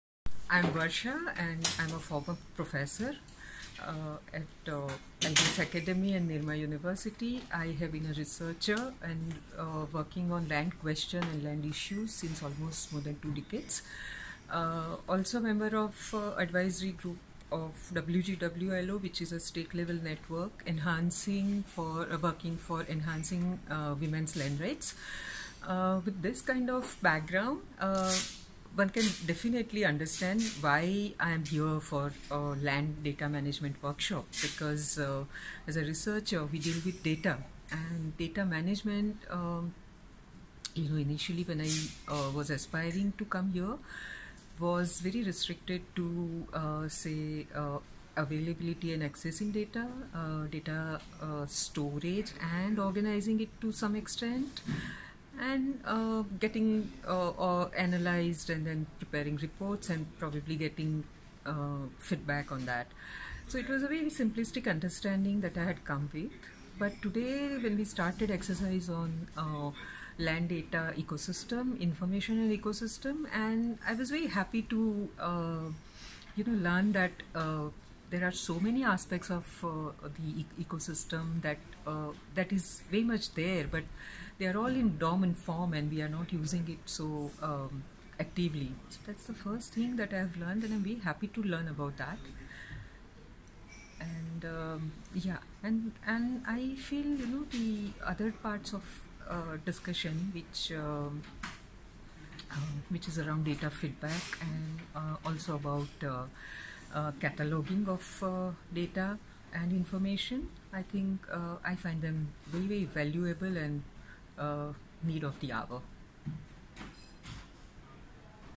Varsha Ganguly Testimonial on the India Land Information Ecosystem Workshop
Varsha Ganguly, convener of the Working Group on Women and Land Ownership (WGWLO) in Gujarat, India, provides her perspective on the India Land Information Ecosystem Workshop.